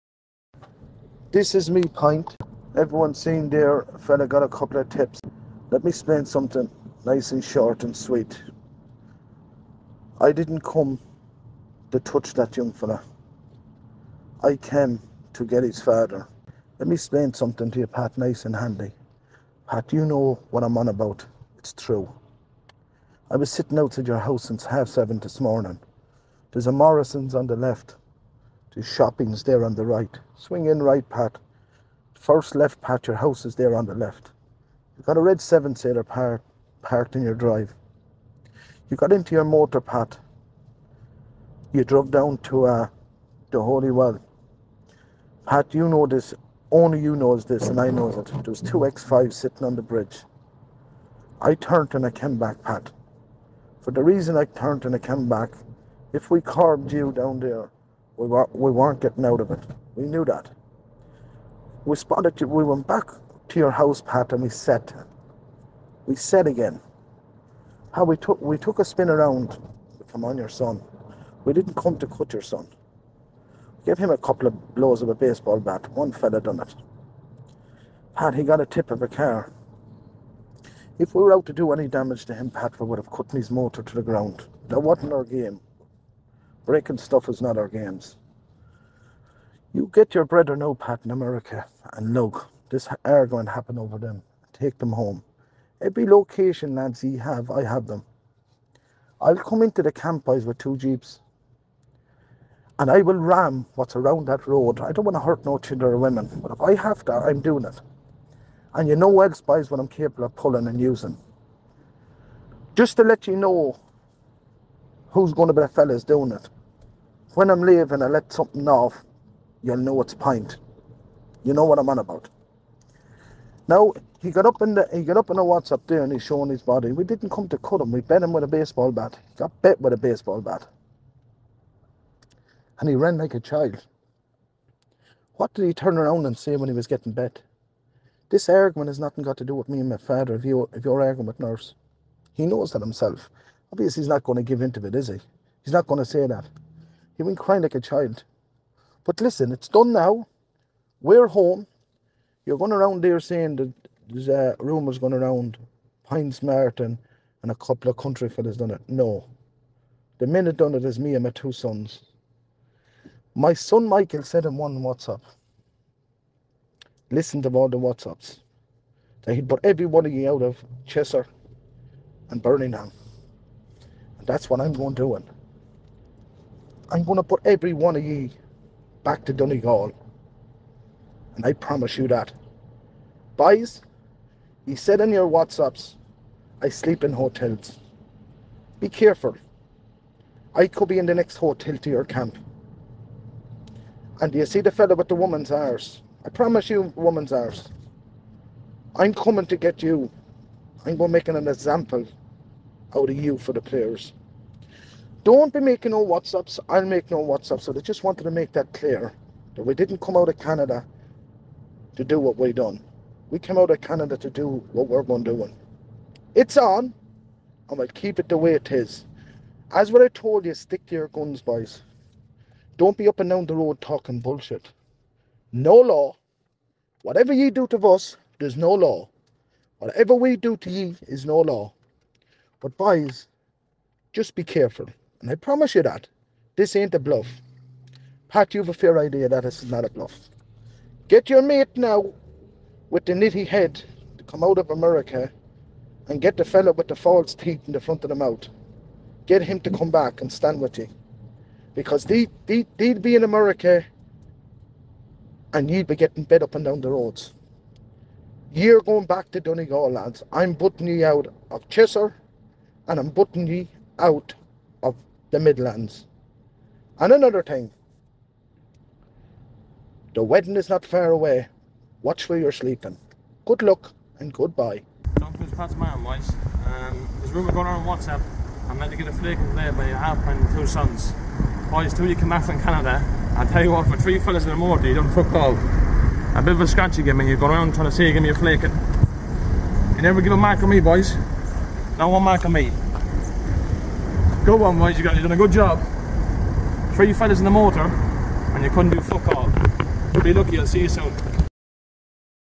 Pat don't think about what we did think about what we're going to do
Irish Traveller half pint message to John player's Pat